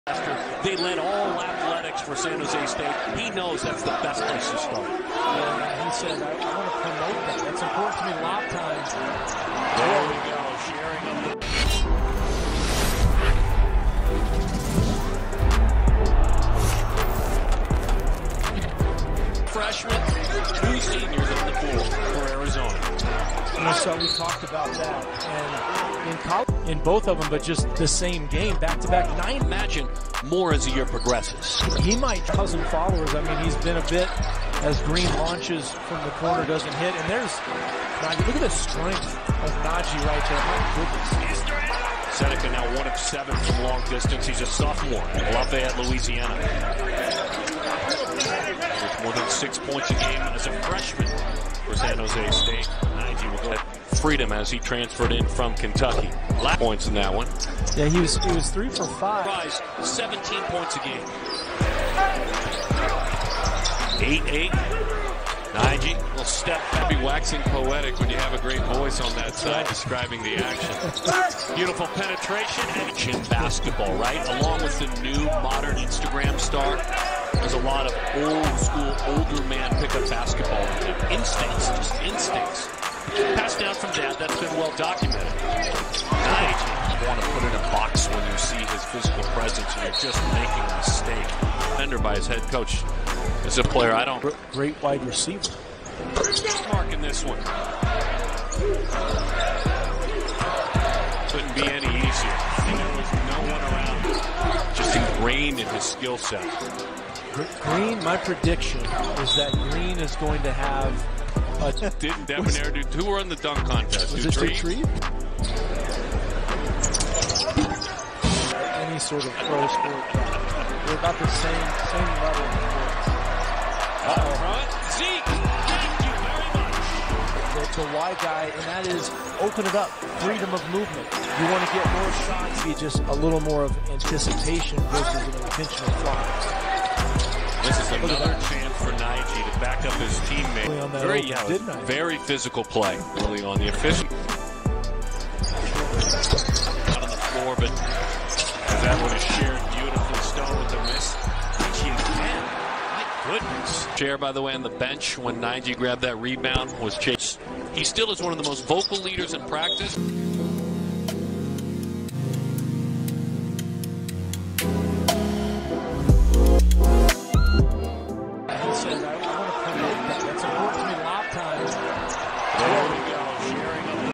Zeke Nnaji Arizona Wildcats 26 PTS 11 REBS 2 AST vs San Jose State | PERFECT GAME!
Zeke Nnaji, who has averaged 21.7 points and 6.7 rebounds per game, while shooting 81.3 percent from the field in three games, praised those around him for setting him up with quality opportunities."My teammates are giving me the ball and the coaching staff have a good offense for us that's been working so far," Nnaji said. "I think we're just continuing to go with the flow." That flow was virtually unimpeded Thursday, with 11 Wildcats scoring points, while dishing out 24 assists to 8 turnovers in the victory. Miller remarked that the Wildcats have an embarrassment of riches, with a deep bench full of experienced players, such as University of California-Irvine transfer, Max Hazzard, who scored six points in the team's win.